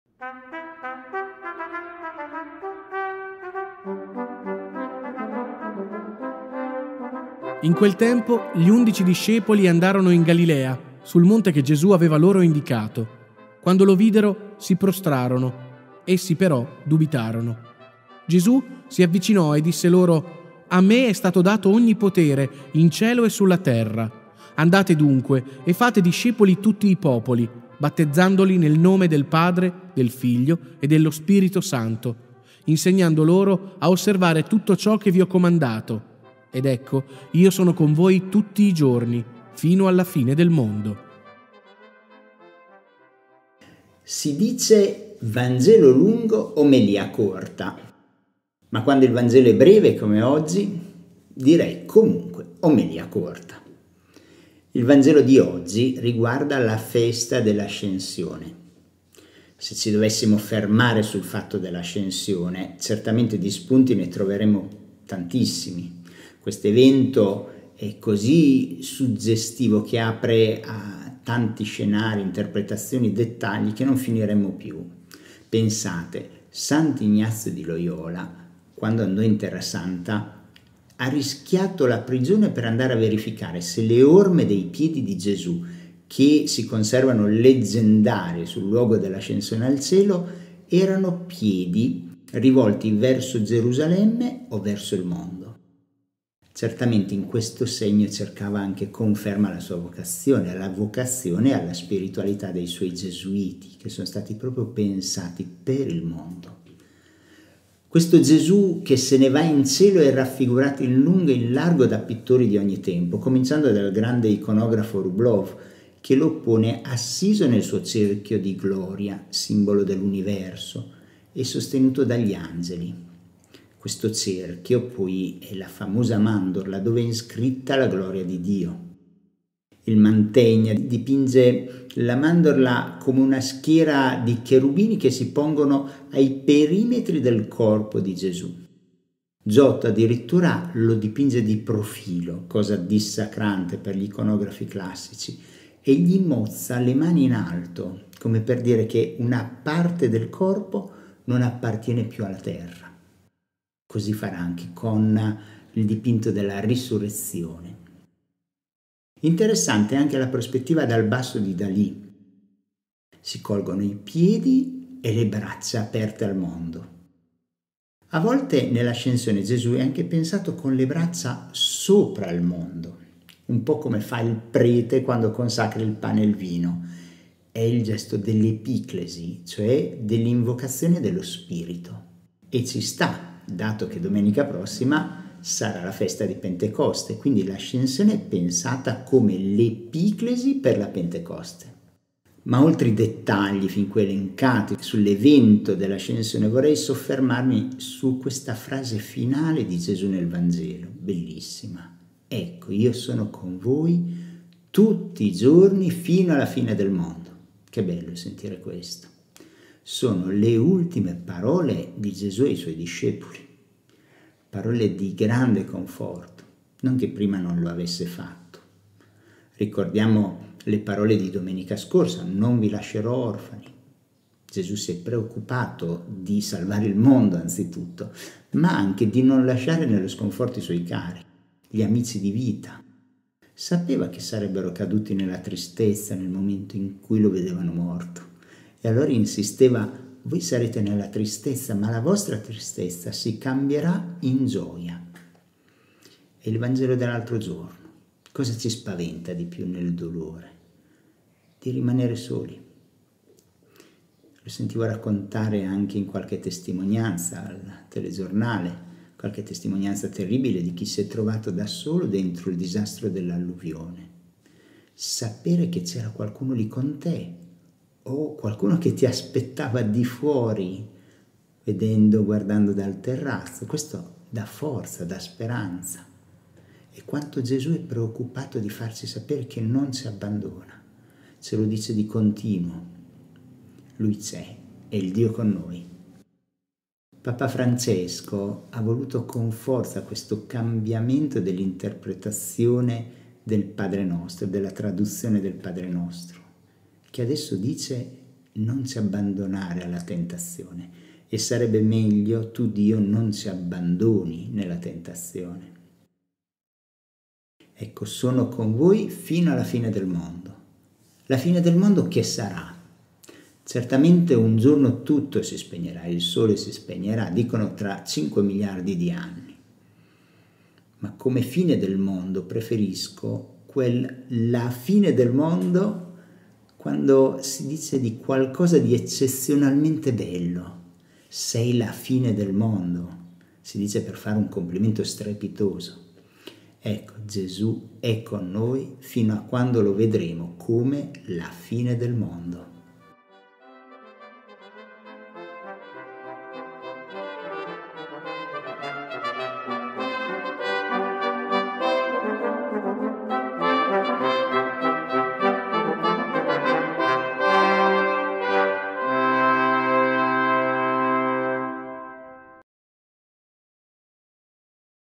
Vangelo del 21 Maggio 2023: Matteo 28,16-20 con il commento di don Franco Mastrolonardo
In quel tempo, gli undici discepoli andarono in Galilea, sul monte che Gesù aveva loro indicato. Quando lo videro, si prostrarono. Essi però dubitarono. Gesù si avvicinò e disse loro: «A me è stato dato ogni potere in cielo e sulla terra. Andate dunque e fate discepoli tutti i popoli, battezzandoli nel nome del Padre e del Figlio e dello Spirito Santo, insegnando loro a osservare tutto ciò che vi ho comandato. Ed ecco, io sono con voi tutti i giorni, fino alla fine del mondo».